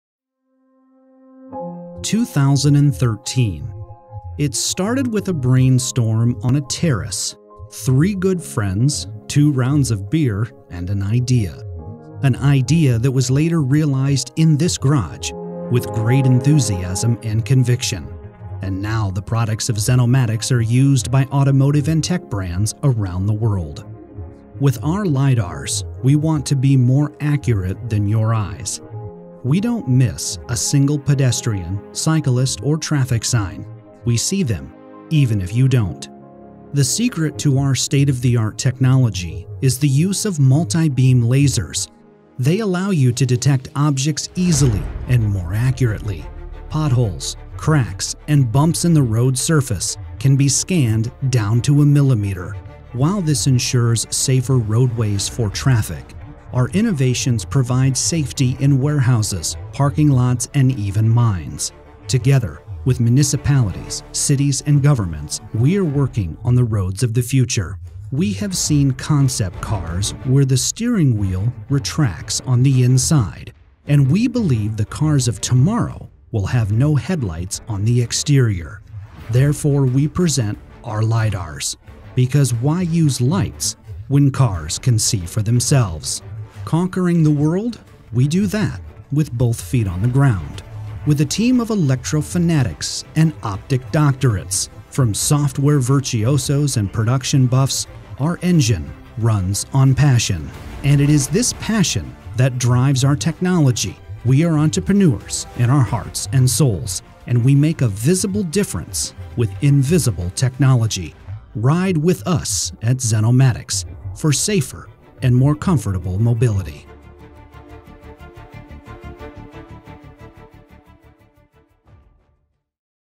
Welcome to XenomatiX' true-solid-state LiDAR
XenomatiX has developed two main lidar sensors:
1. XenoLidar-X is a TrueSolidStateLidar for automotive, ADAS, and industrial applications, with excellent performance and scalability.
2. XenoTrack, millimeter accurate short range lidar used to measure road characteristics like international roughness index and rutting index; simulations applications, pro-active suspension, and road classification.